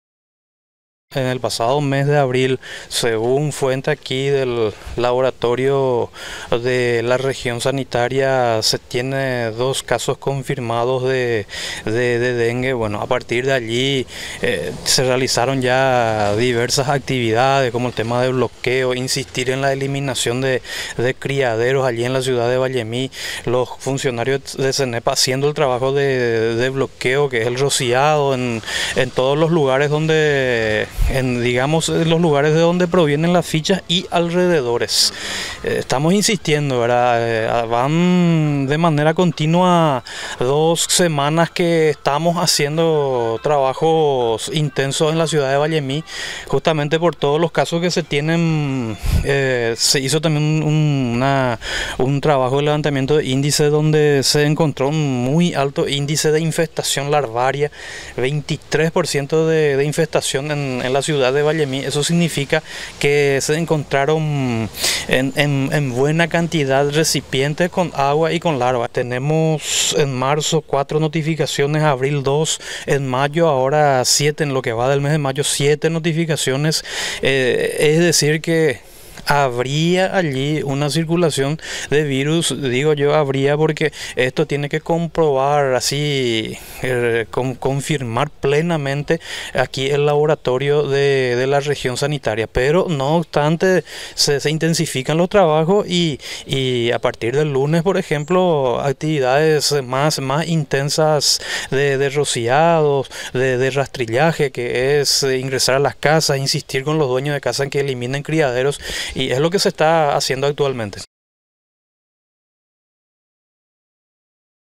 ALERTA ANTI DENGUE EN VALLEMI Daniel Herrera
VALLEMI EN ALERTA POR DENGUE
El Senepa reforzará los controles de criaderos en Vallemí, este mes fueron reportados 7 casos sospechosos.